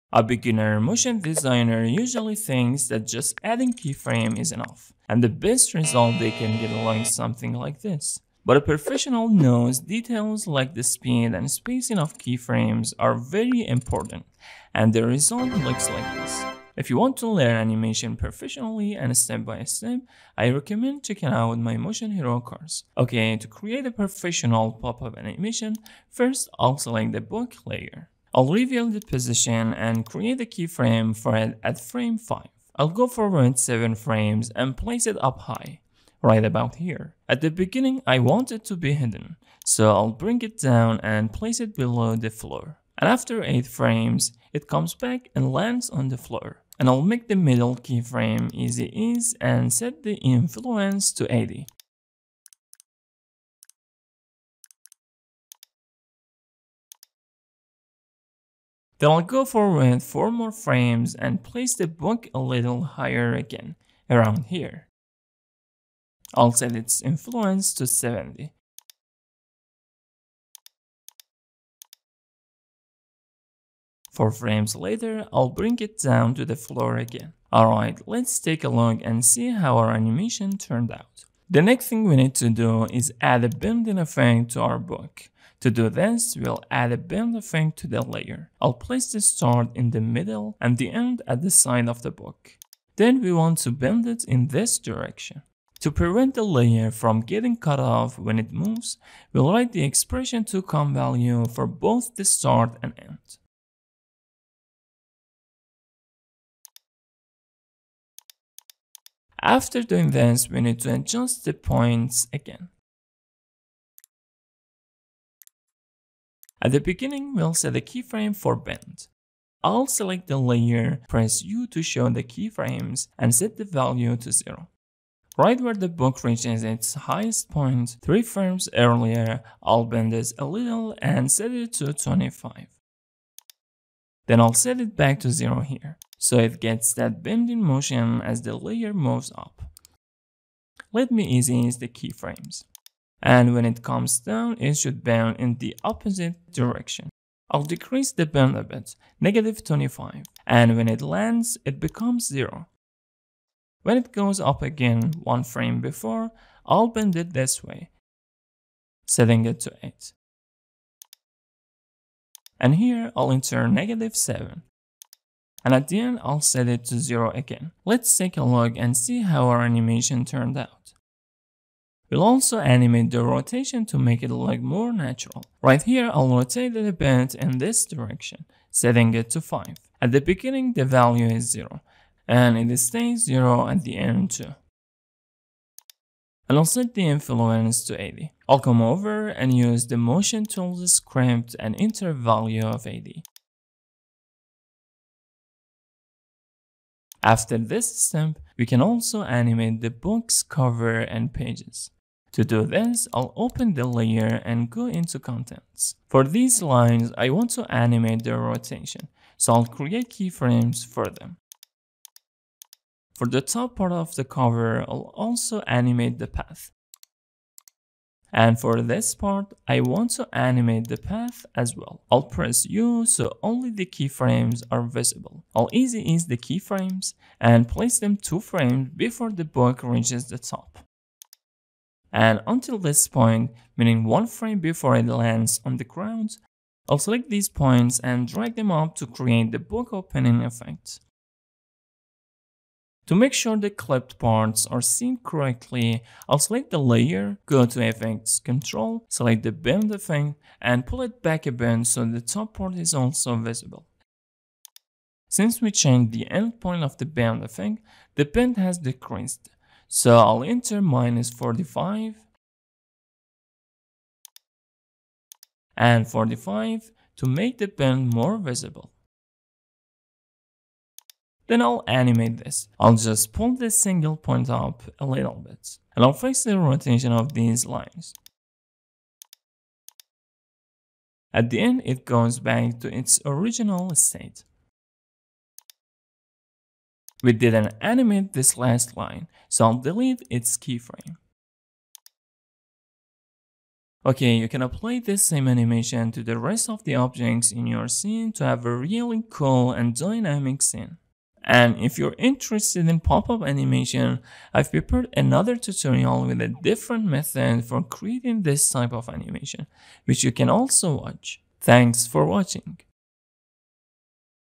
You Loved Part 1! Let's Level Up Your Pop Up Animations in After Effects | Part 2
Ready to Level Up your Pop-Up Animations in After Effects? In this Part 2 tutorial, we dive into advanced techniques that will make your motion smoother, snappier, and more professional for every project.

🚀 Click here to access my comprehensive and advanced After Effects Course

What We Cover in This Advanced Tutorial (Part 2):
Building on the success of Part 1, we’ll look at the key details and pro tricks that elevate your pop-up animation from basic to truly professional.

Technique 1: Professional use of the Keyframe Velocity for buttery smooth motion.

Technique 2: Adding controlled Overshoot and Bounce for a dynamic, weighty look.

Technique 3: A simple Keyframe trick that will dramatically speed up your workflow.

This video is specifically designed for my subscribers who loved the first part and are now ready to deepen their skills and master the nuances of motion graphics.